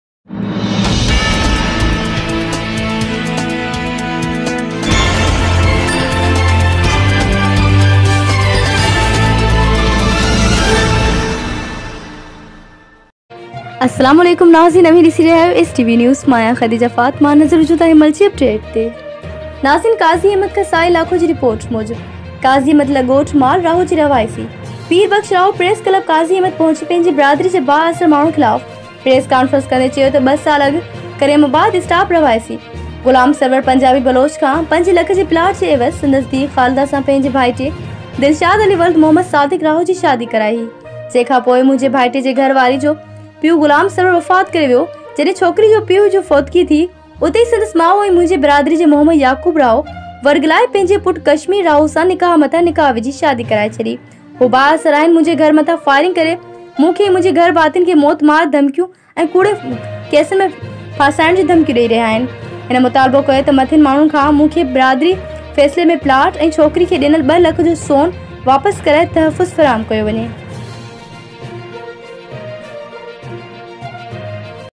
قاضي احمد = ڳوٺ مال راهو جي رهواسي جي بااثر خلاف پريس ڪانفرنس رپورٽ = سائل لاکو
قاضي احمد = ڳوٺ مال راهو جي رهواسي جي بااثر خلاف پريس ڪانفرنس
رپورٽ = سائل لاکو